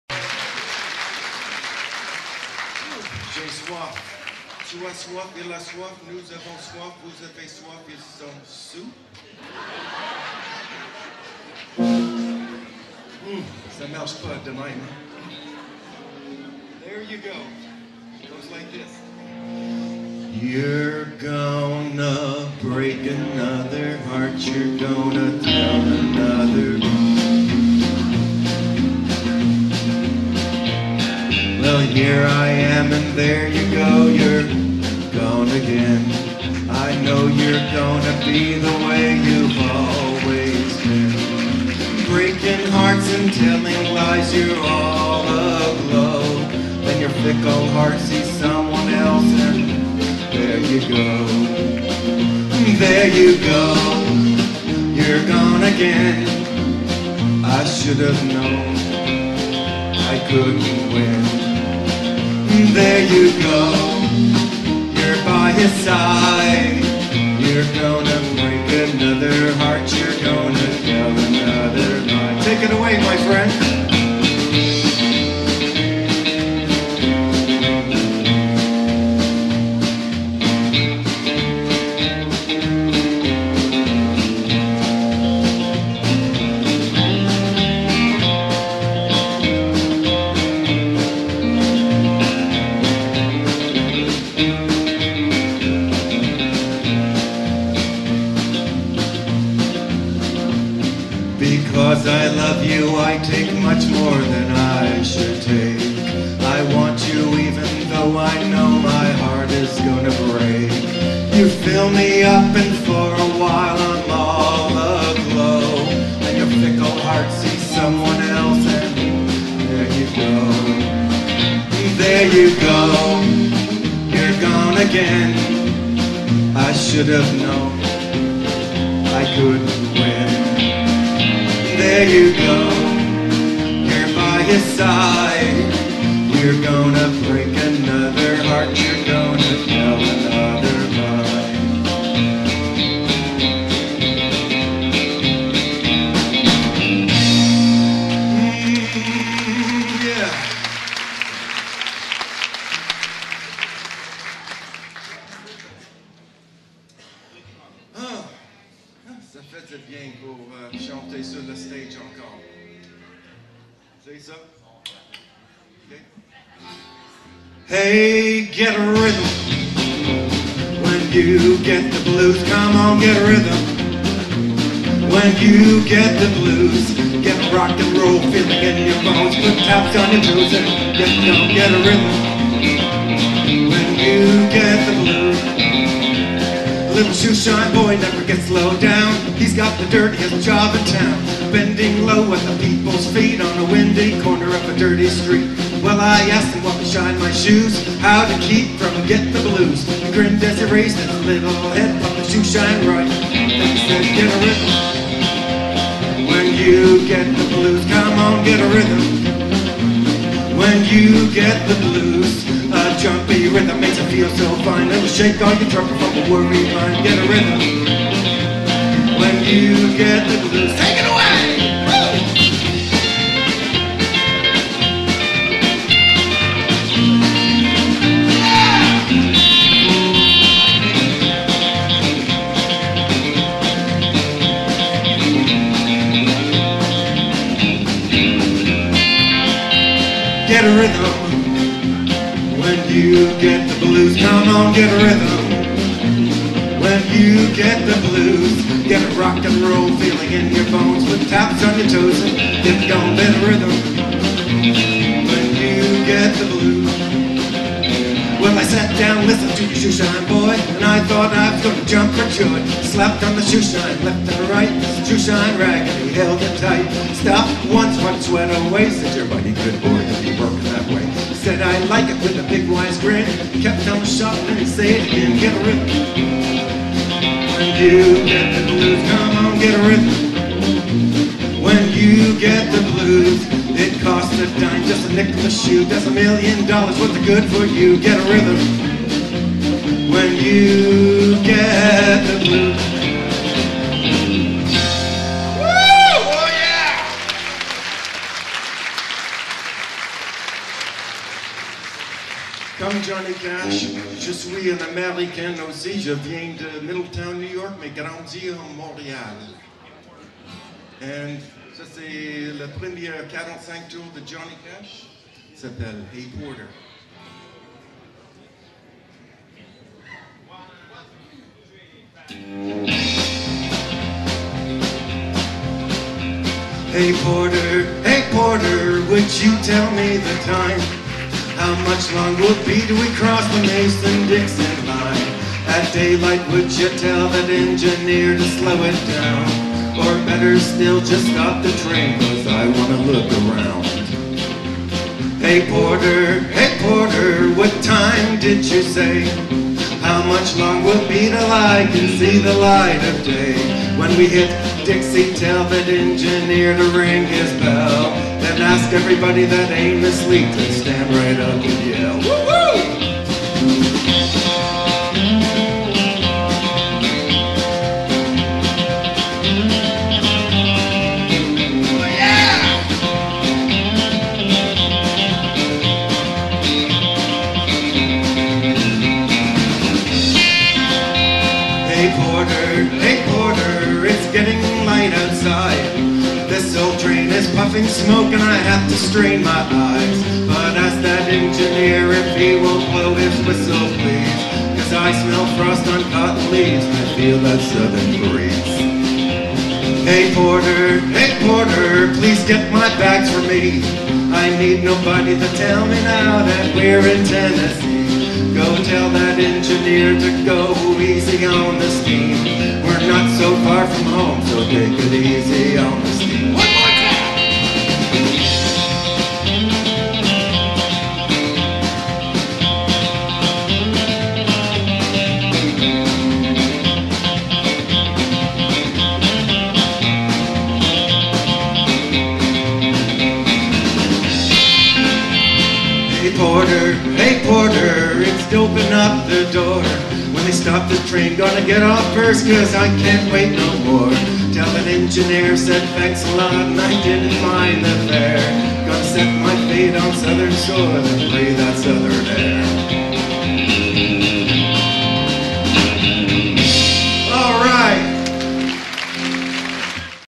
Yamachiche June 22 2013 Part 2
This is the middle part of the show of my second year performing at Yamachiche country festival which is always the 3rd weekend of June every year!!